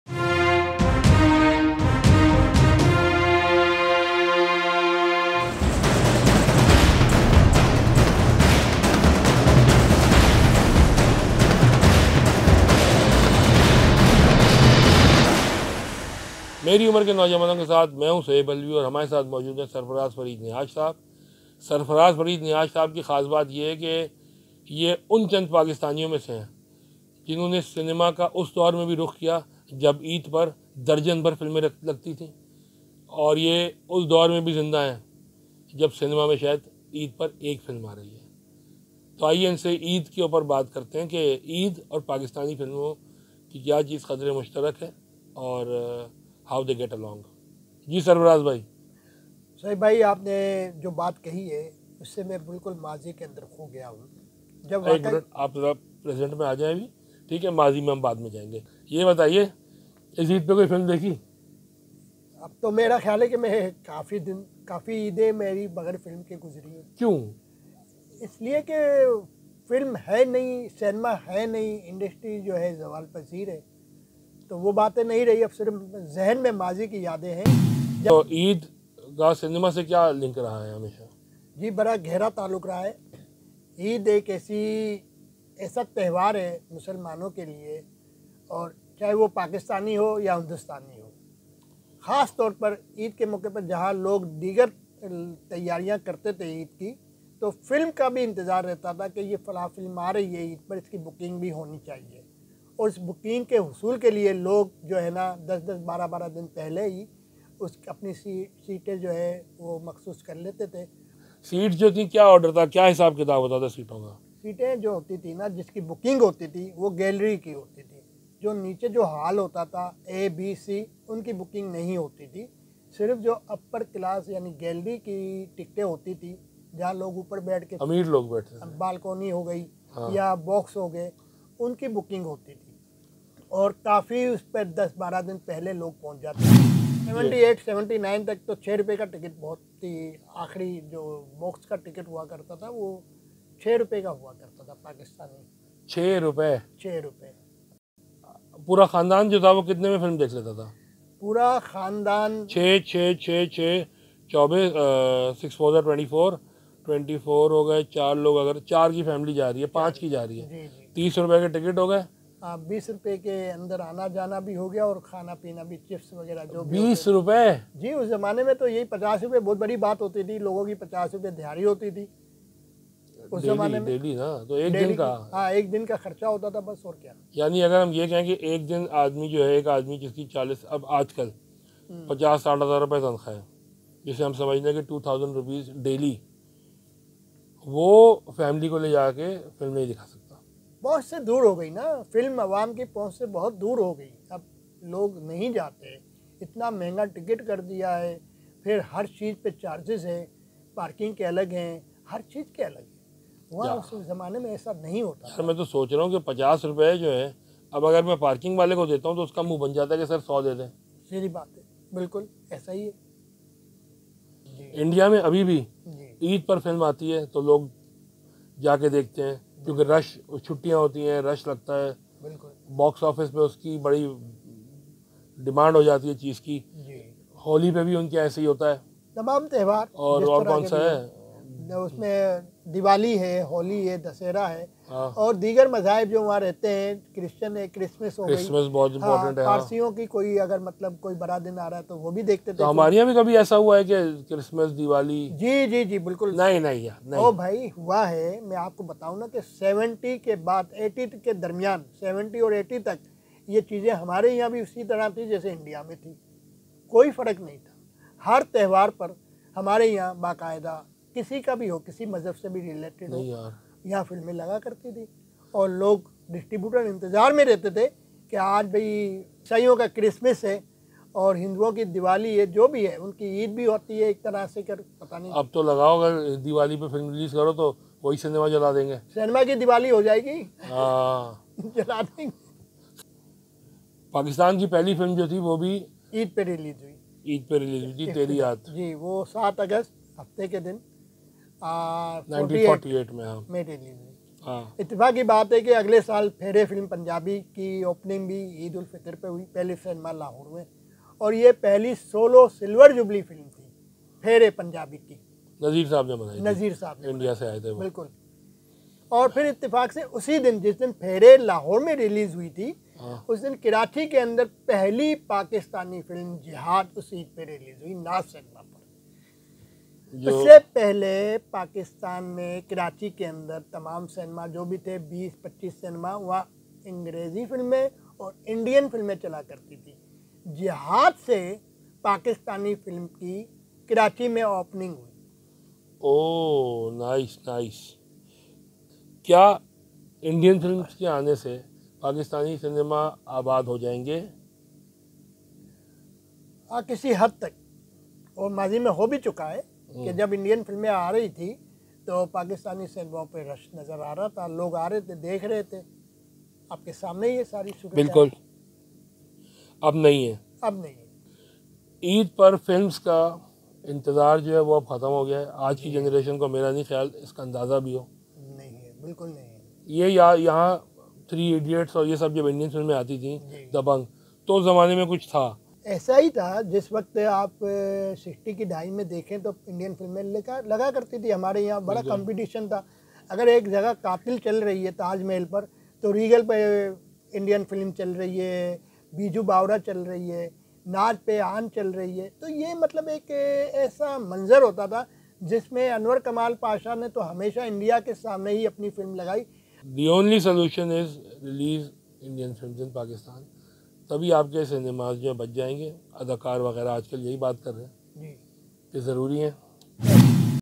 Eid & Movie connection.
I discuss Eid & movies of the yore with film historian Sarfraz Farid Nihash. It takes viewers back in time to the days when a cinema ticket would cost as much as six rupees.